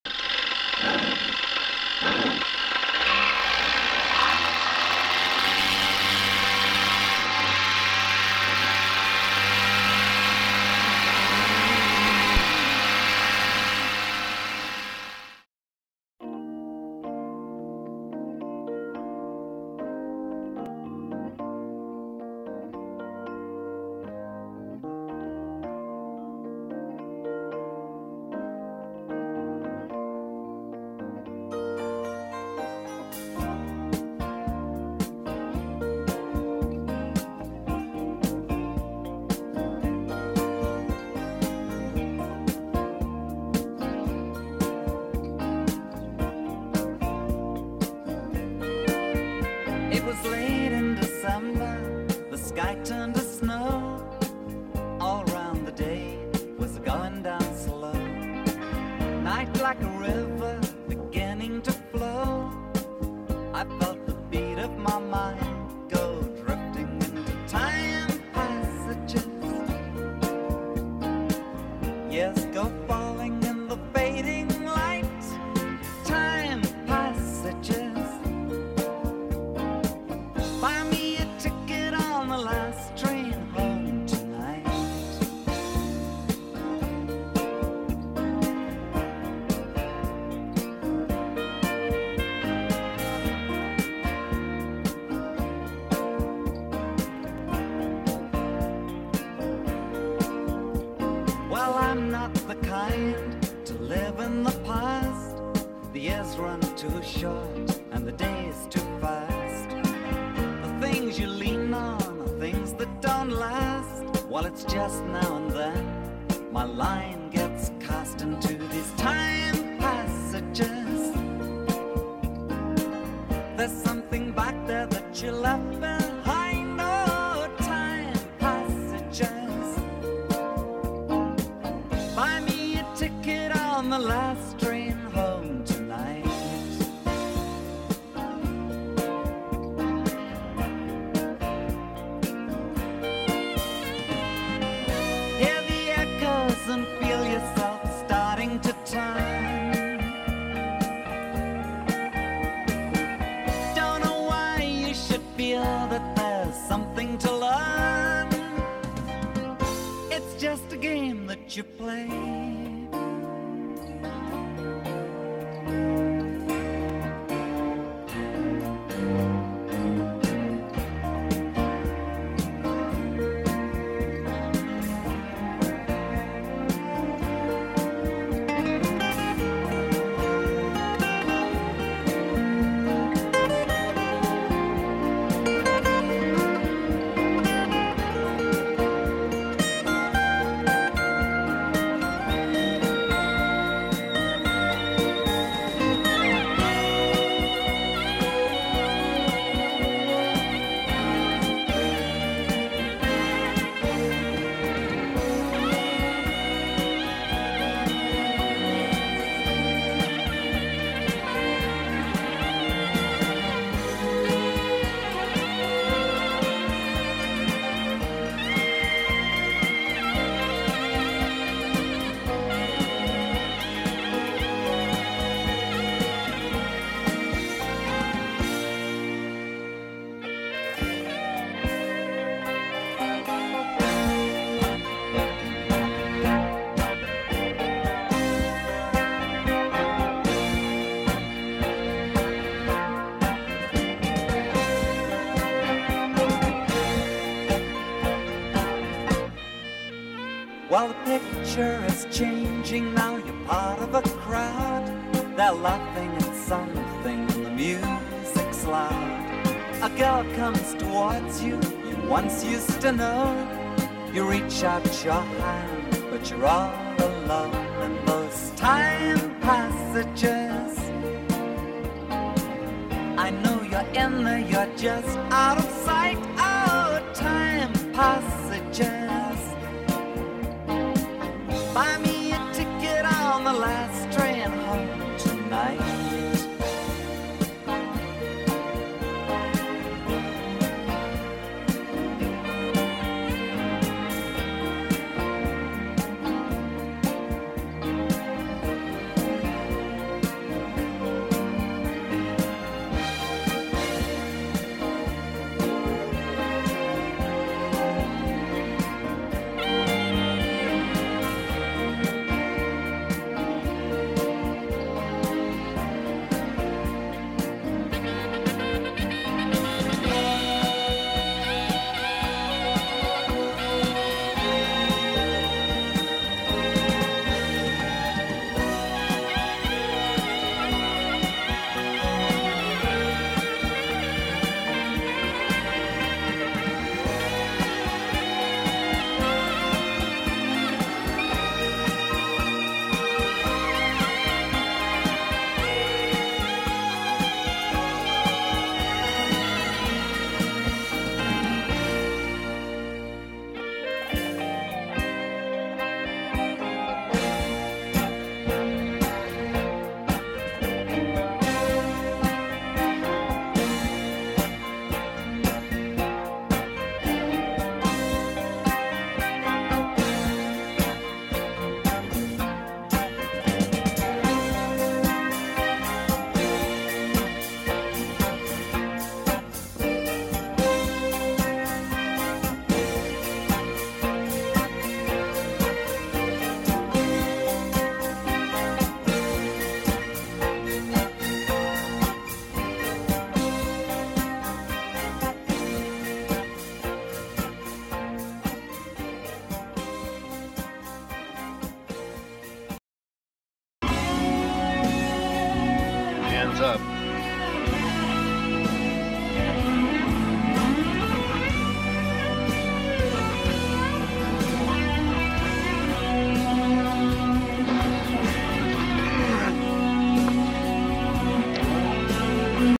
Finally Made It To Lake Mead
I finally flew to Lake Mead. Passed between the mountains and Boulder City. It was stunningly beautiful. Nice and warm above 300 feet (AGL). Stumbled upon the "Rage Triathlon" not sure why they call it that?